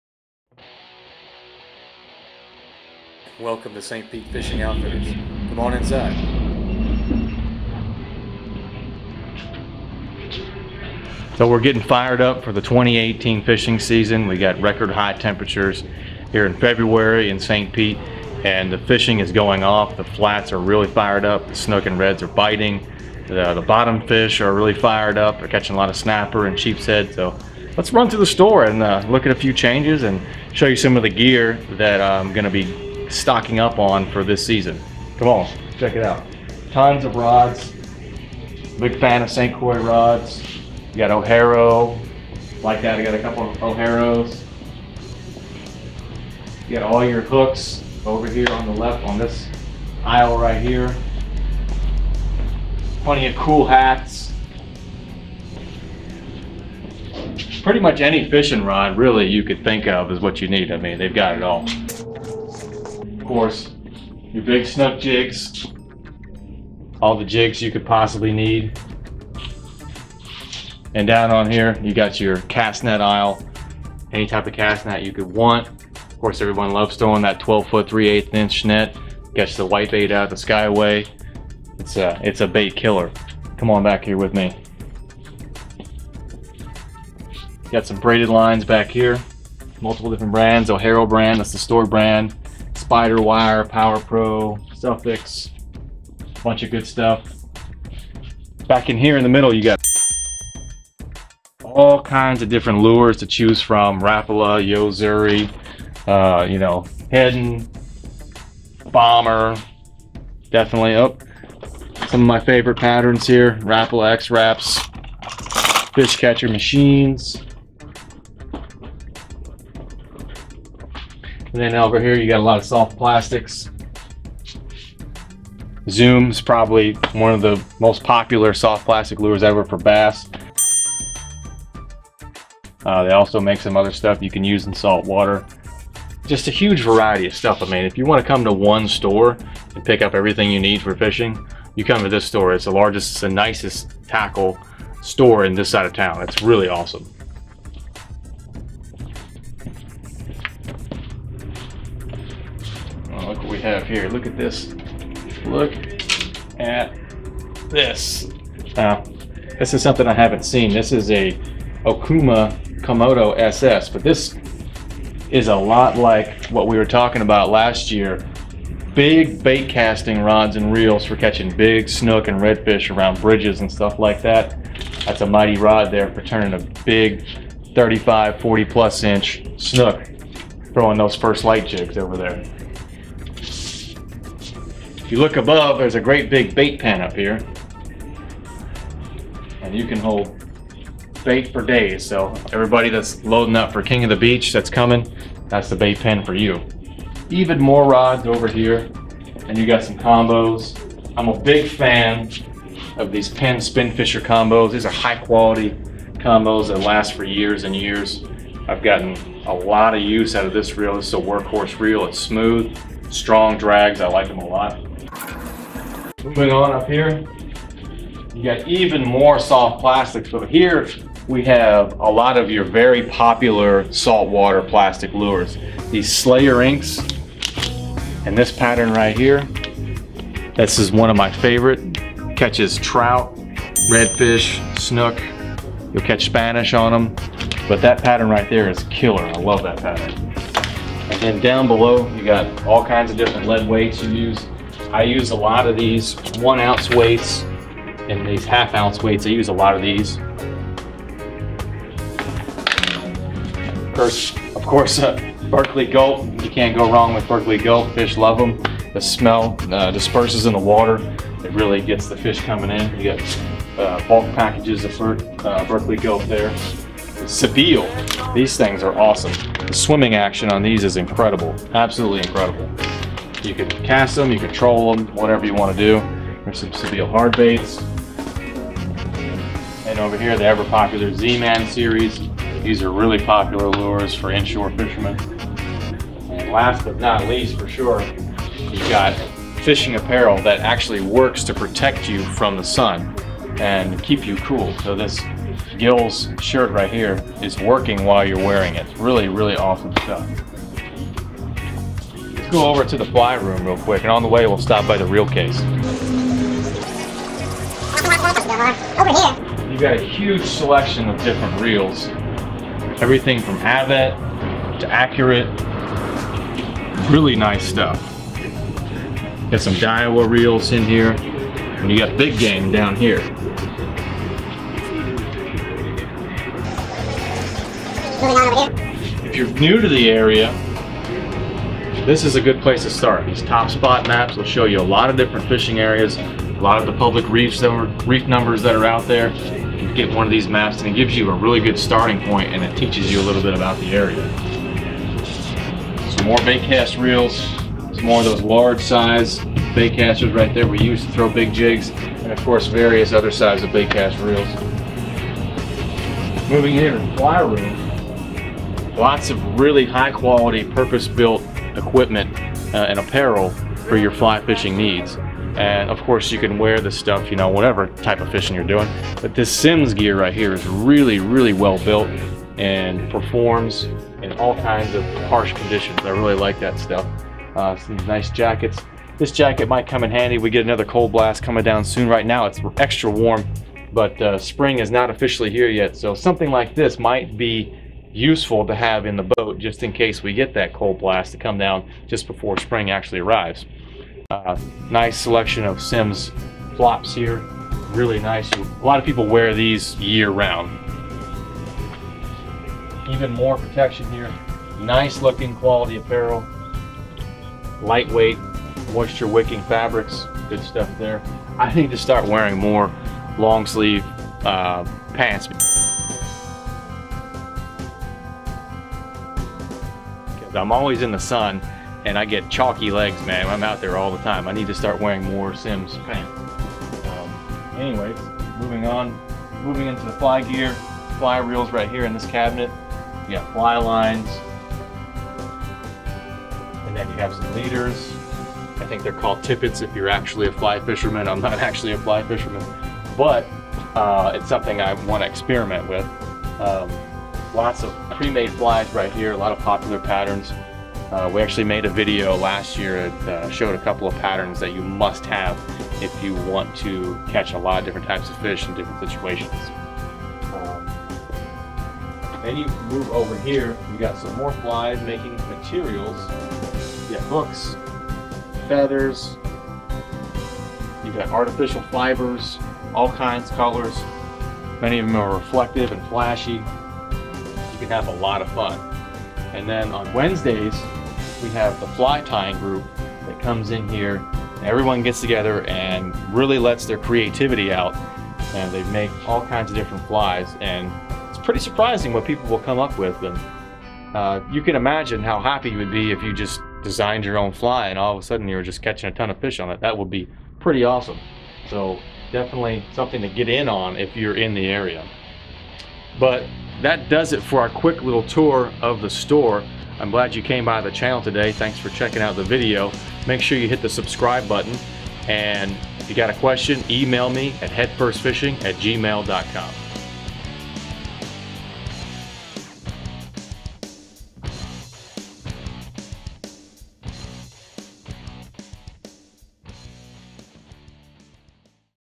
2018 Tour De Store - Saint Pete Fishing Outfitters Quick Tour
2018 Spring Fishing has begun. Step inside Saint Pete Fishing OUtfitters and see a sampling of great gear available for your Head First Fishing Adventures. We have all the gear you need for snook, redfish ,trout, snapper, sheepshead, tarpon, triple tail, grouper, cobia, and more!

Don't miss our fly tying class on wednesdays! Its a great group of guys that are dedicated to learning the art of tying flies.